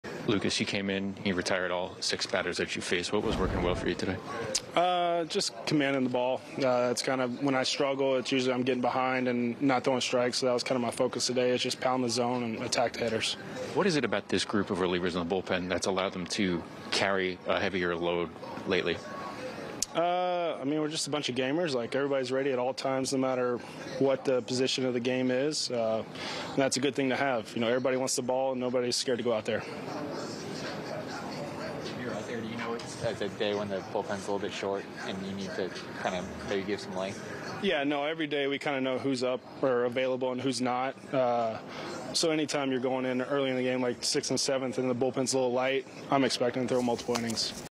Lucas Luetge works perfect innings out the pen vs. Oakland
Lucas Luetge sent down six straight A's Wednesday and has only allowed 1 run over his last 11.2 IP.